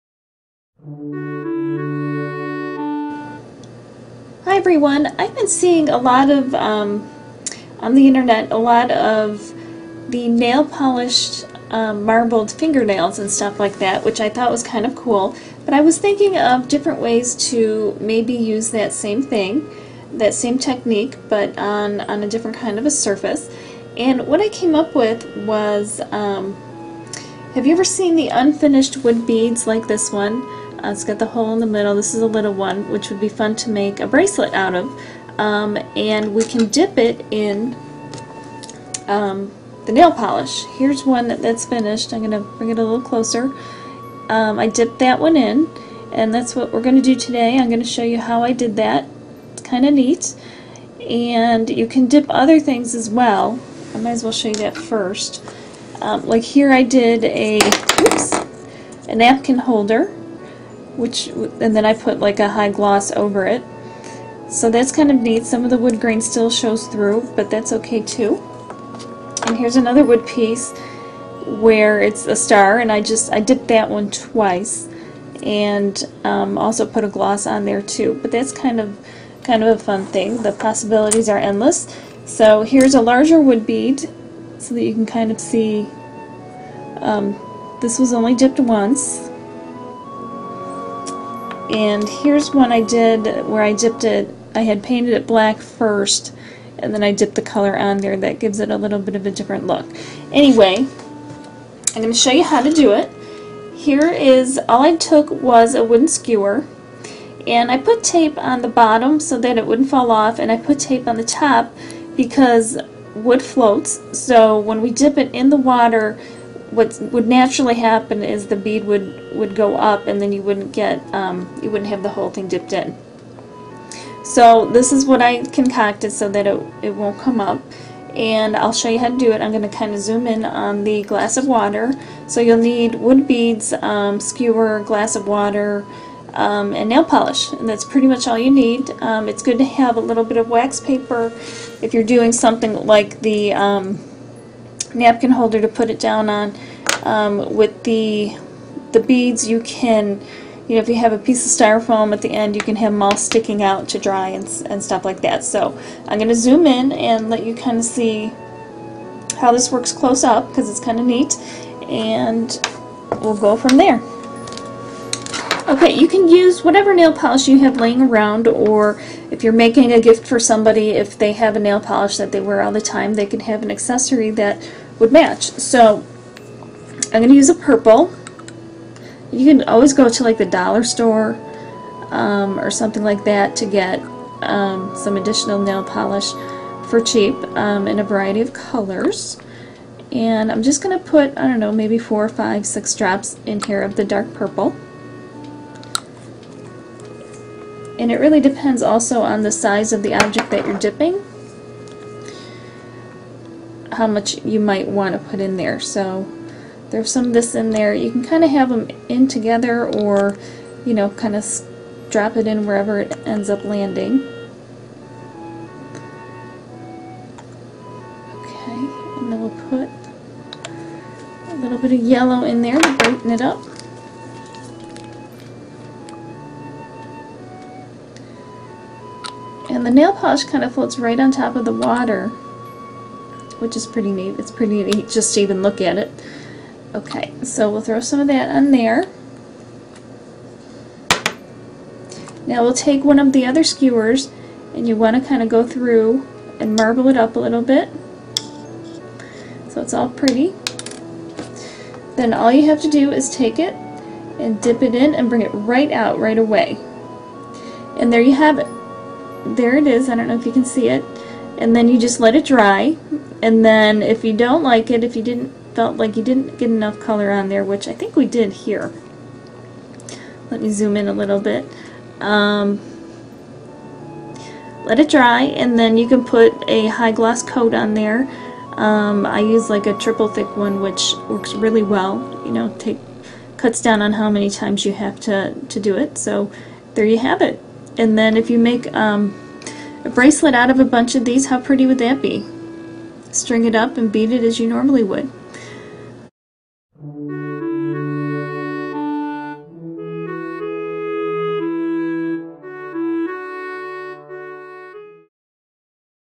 Nail Polish Marble Dipped Beads
Use nail polish to create marbled beads.  Fun and easy craft!